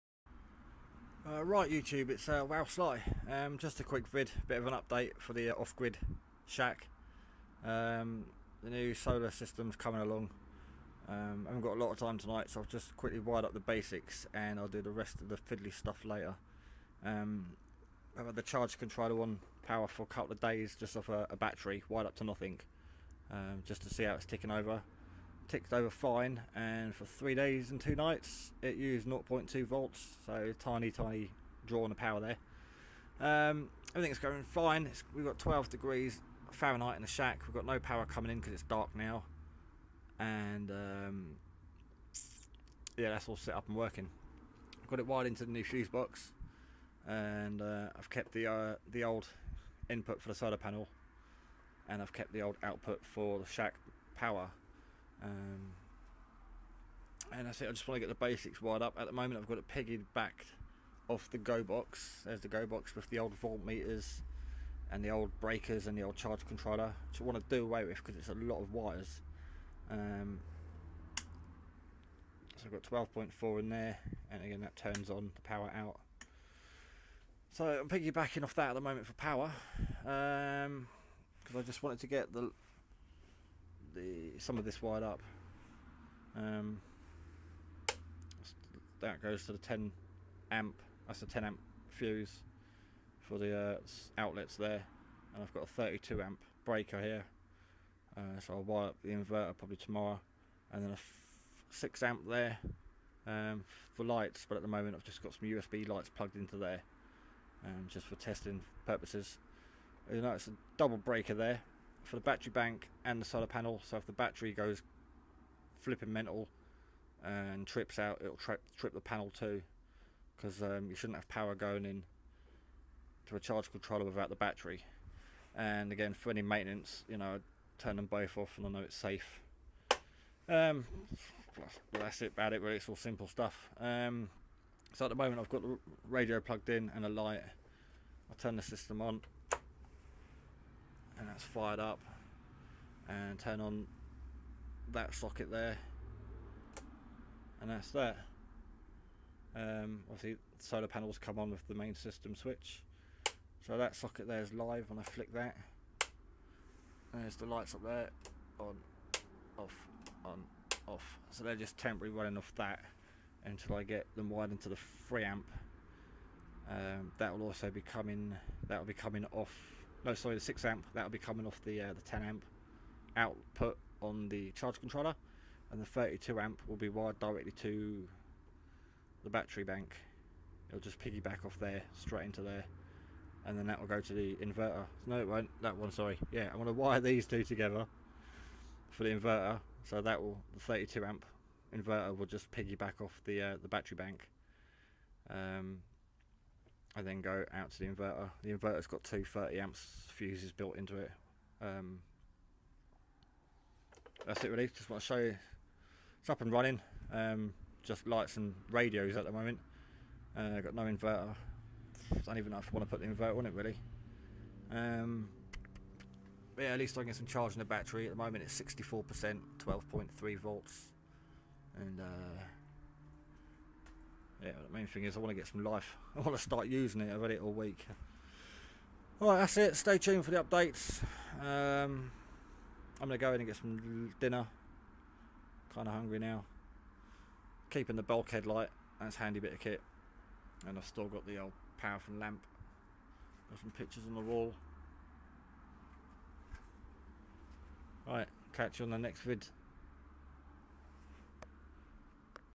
off grid solar shack re build
built as a hobby to run lights and radios in the shack off grid.
just a bit of fun but can all so be took camping or any where else as the hole thing slides off the wall.
great for power cuts :D
follow the link to my face book group for homemade solar projects in the uk